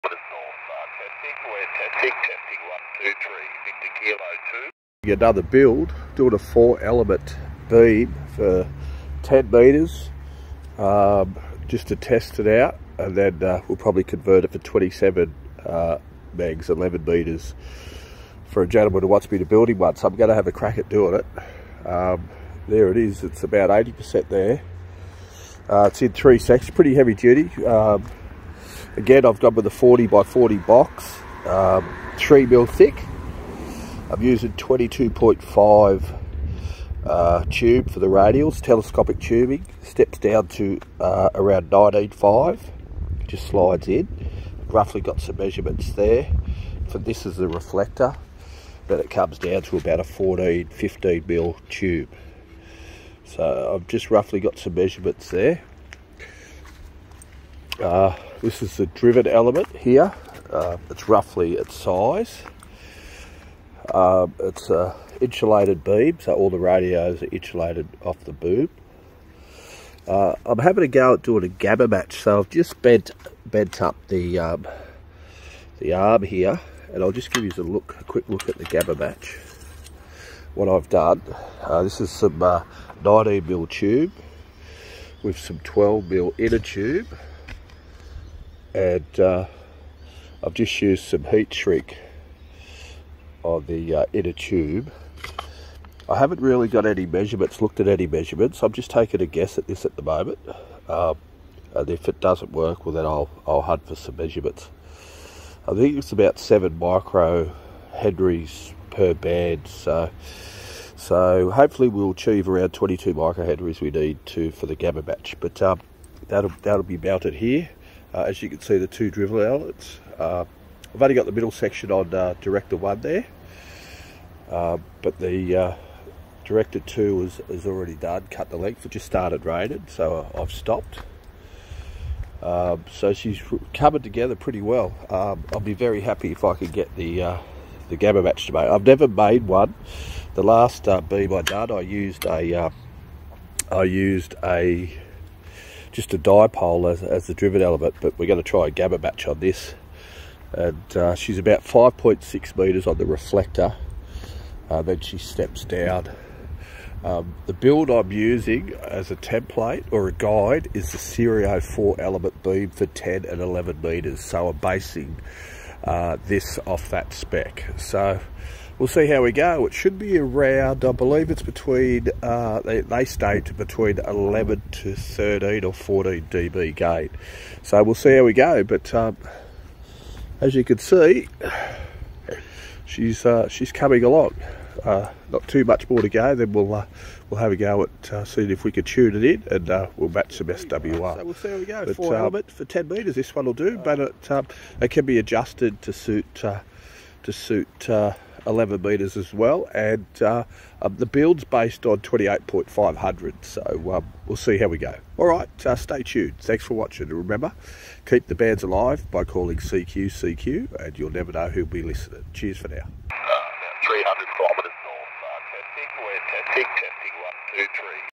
MY DIY HF ANTENNA BUILD 4 ELEMENT FOR 10 METERS PT1
Well at it again my new build is a 4-element for 10 meters, with a gamma match, based on the SIRO 4-element beam expected to gain 11-13.15dB gain.

She looks good so far so let's see how it goes, I have been asked to build a 4-element for 11 meters so I thought I try a meter one 1st

Ohh KEEP THE BANDS ALIIVE BY CALLING CQ CQ

73's